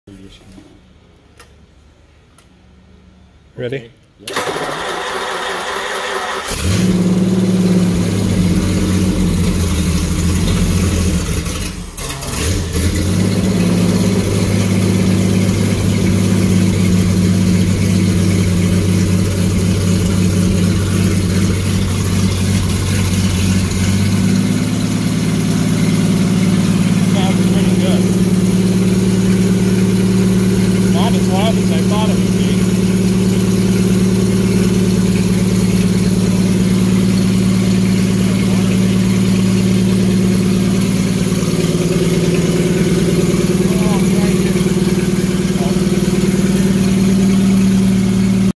Cold start a restored Seagrave V12 gas dual plug fire truck engine
Cold start of a restored Seagrave V12 gasoline dual spark plug fire truck engine